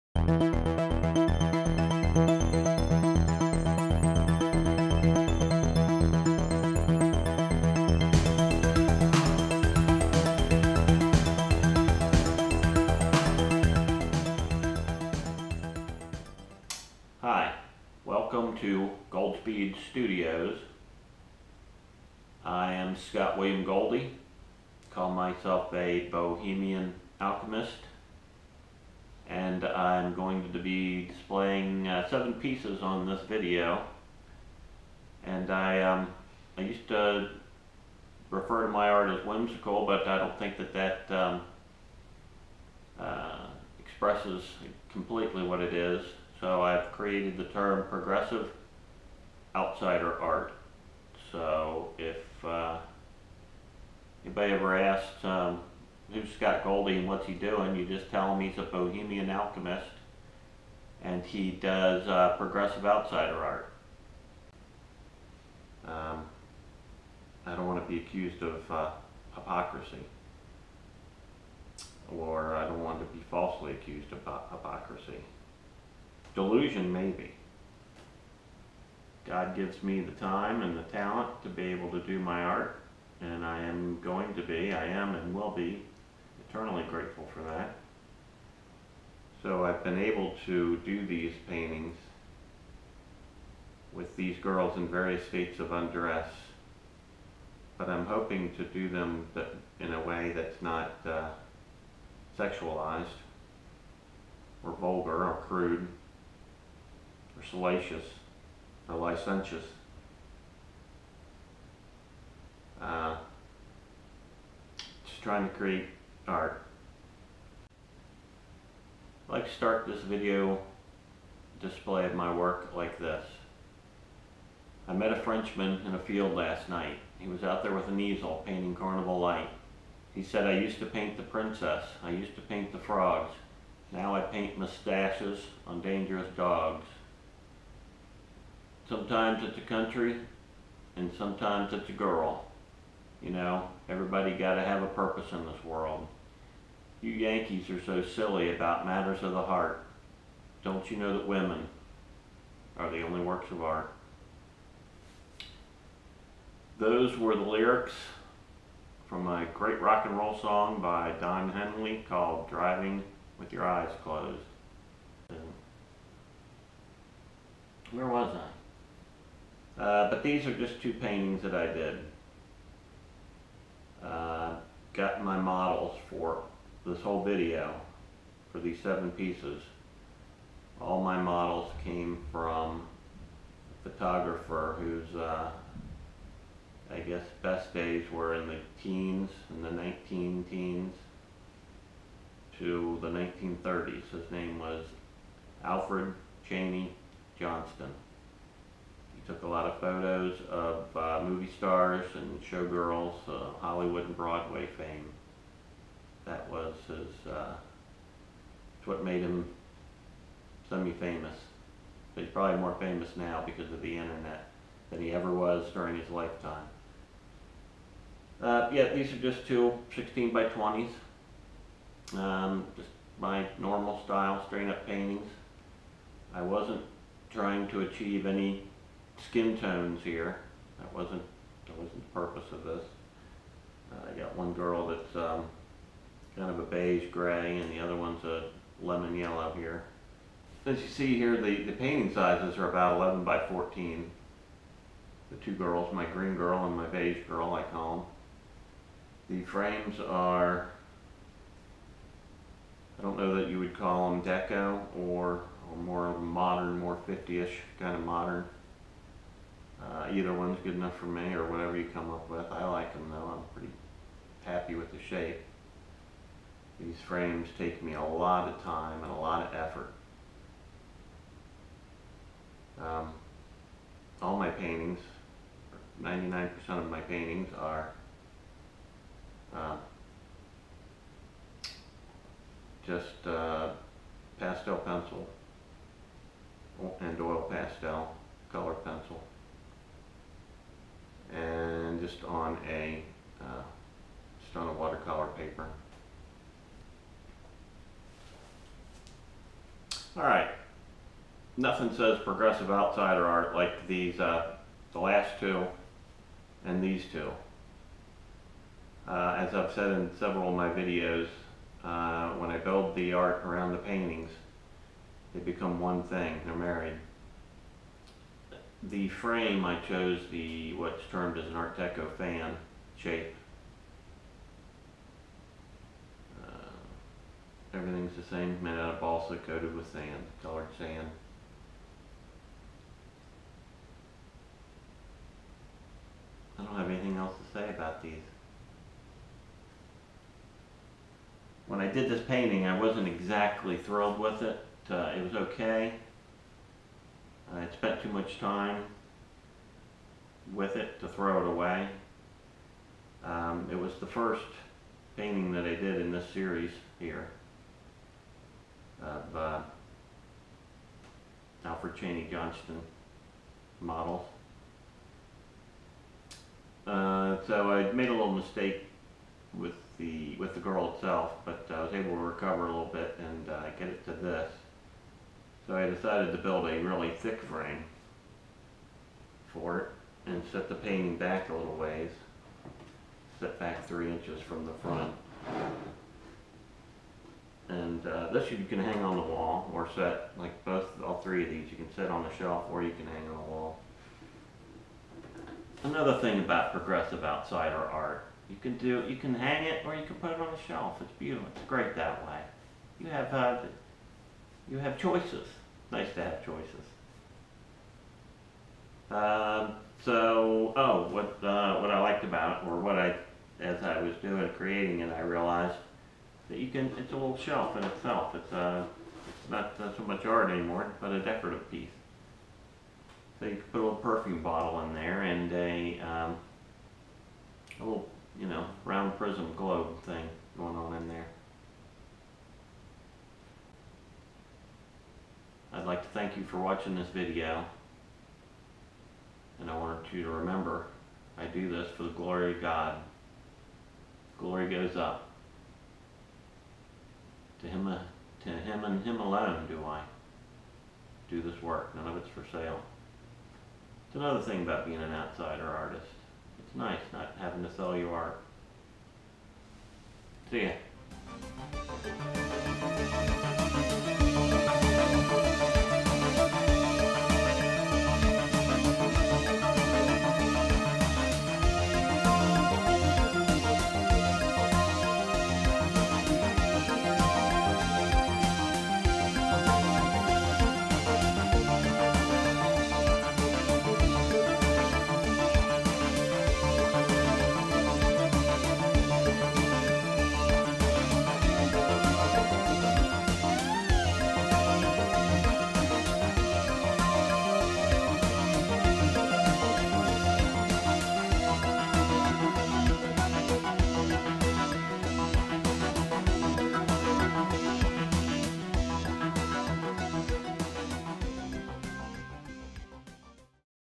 Johnston' girls by Scott William Goldy
A short video from Scott William Goldy exhibiting 7 pieces done in the first half of 2015. Scott William Goldy: Painter - Sculptor  – Writer - bohemian alchemist- for God’s Glory